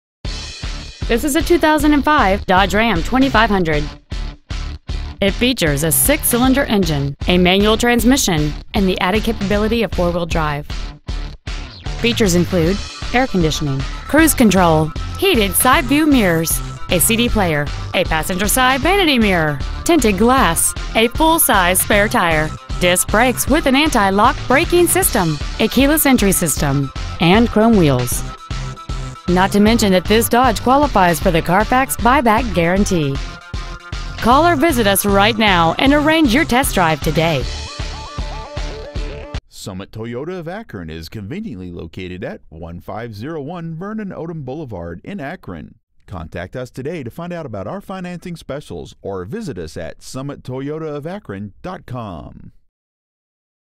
Used 2005 Dodge Ram 2500 Akron OH 44320
Year : 2005
 Make : Dodge
 Model : Ram 2500
 Engine : Diesel I6 5.9L/360
 Trans . : Manual
 Exterior : Bright White
 Miles : 169,008
 Interior : Dark Slate Gray
 Stock : P1150A